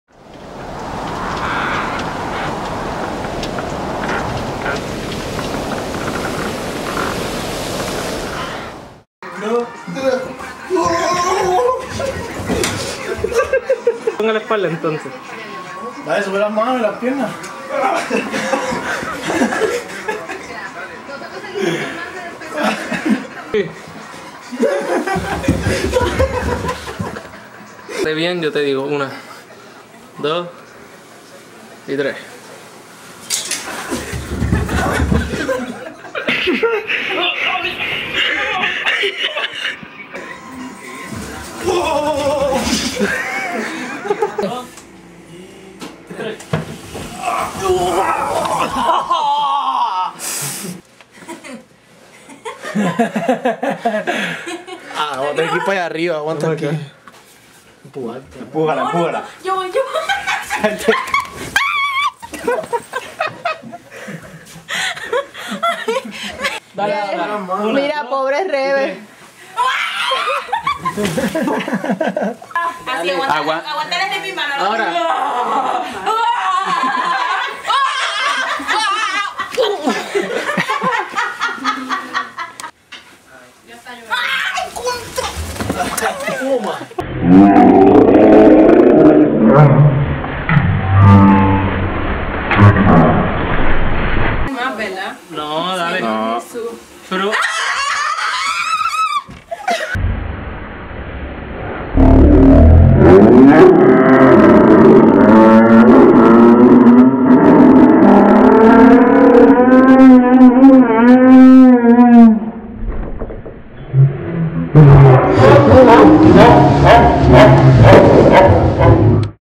bajando por las escaleras de abnel
par de personas que no tenian nada que hacer y desidieron tirarce por unas escalesras en casa de Abnel en California